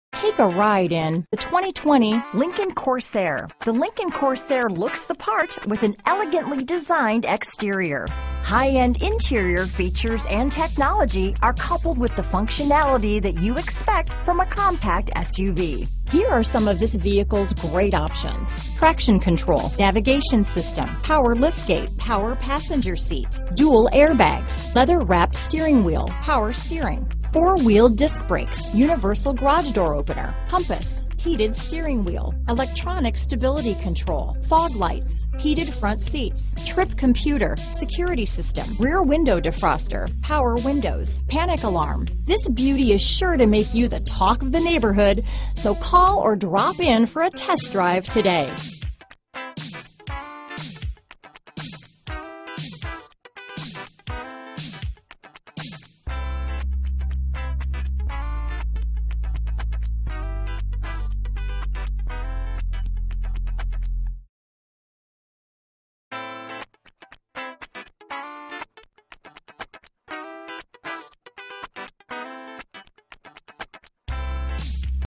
2020 Lincoln Corsair Louisville, Lexington, Elizabethtown, KY New Albany, IN Jeffersonville, IN 10L2
Magnetic Gray New 2020 Lincoln Corsair available at Oxmoor Ford in Louisville, Kentucky. Servicing the Lexington, Elizabethtown, KY New Albany, IN Jeffersonville, IN area.

Oxmoor Ford
100 Oxmoor Ln

 Price includes: $1000 - Retail Bonus Customer Cash. Exp. 11/03/2020